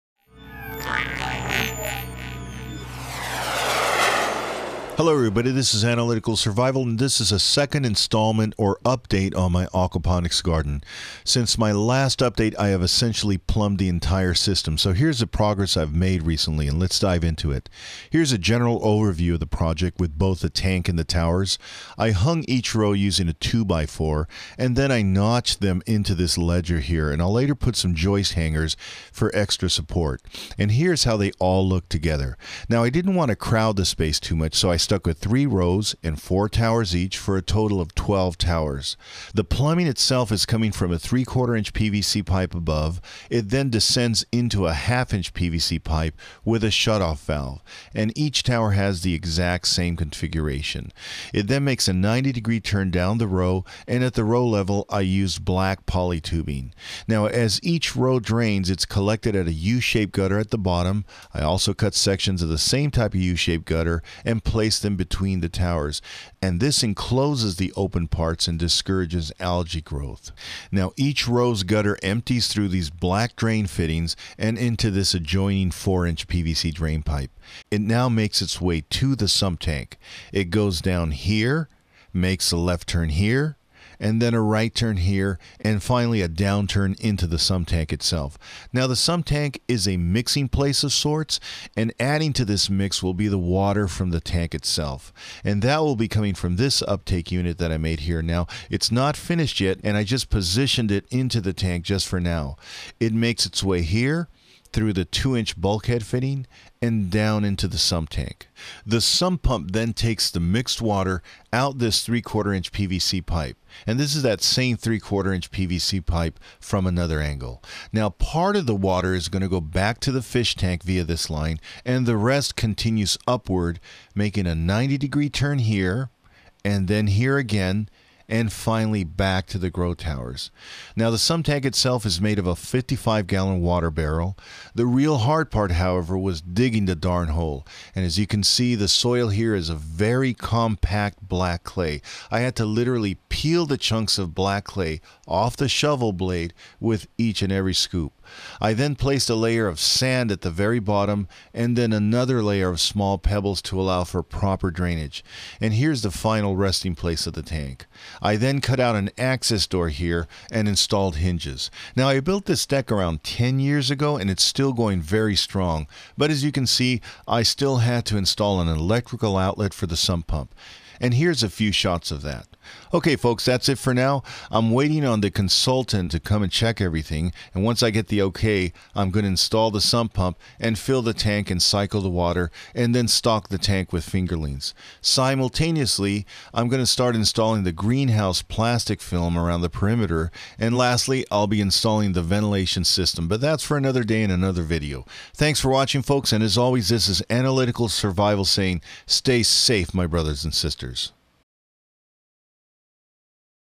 Aquaponics Garden - Update #2
A quick update on our Aquaponics Garden Project.  Enjoy!  --GM

Aquaponics is a form of gardening that recycles water within a closed system.  The most basic definition is that the fish in the tank defecate and urinate.  This defecation/urination-laden water is carried to the plants.  The plants in turn clean the water and absorb the fertilized water as nutrients for growth.  The clean water is then returned to the fish tank.  Both fish and plants live in a symbiotic environment - one sustaining the other.